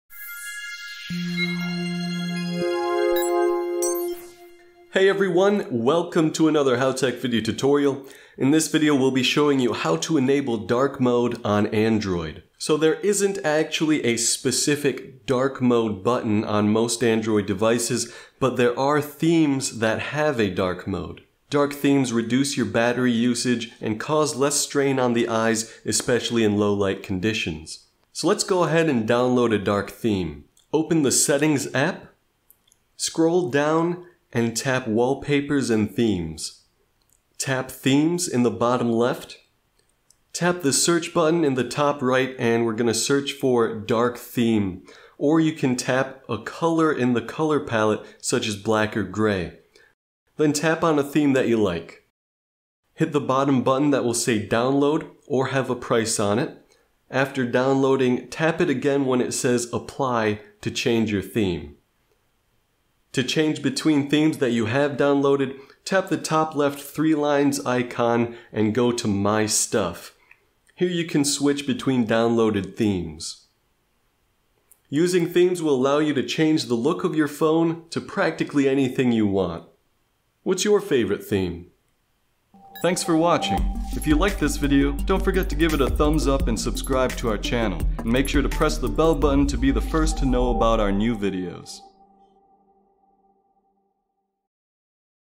How to Enable Dark Mode on Android | Without Third-Party Apps
In this video, we’ll be showing you how to enable dark mode for Android.

There isn’t a specific “dark mode” button on most Android devices, but there are Themes that have a dark mode. Dark Themes reduce your battery usage and cause less strain on the eyes especially in low-light conditions. Let’s go ahead and download a dark Theme.

1. Open the Settings app.
2. Scroll down and tap Wallpapers and themes.
3. Tap Themes in the bottom-left.
4. Tap the search button in the top-right and search for “dark theme” or tap a color in the color palette such as black or gray. Then tap on a Theme that you like!
5. Hit the bottom button that will say “Download” or have a price on it. After downloading, tap it again when it says “Apply” to change your Theme.
6. To change between Themes you have downloaded, tap the top-left three lines icon and go to My Stuff. Here you can switch between downloaded Themes.

Using Themes will allow you to change the look of your phone to practically anything you want!